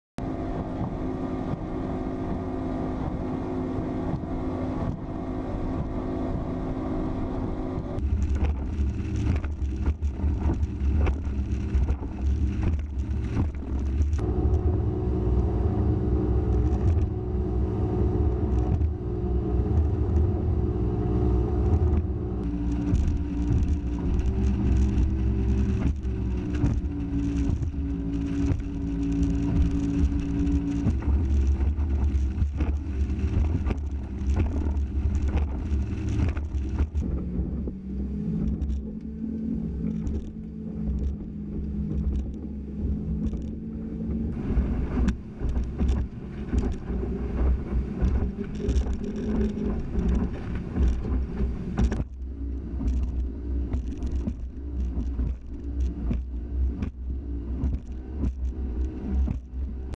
WavePro in boat view
See the WavePro 2.0 Pedestals in action hitting the waves on Lake Mille Lacs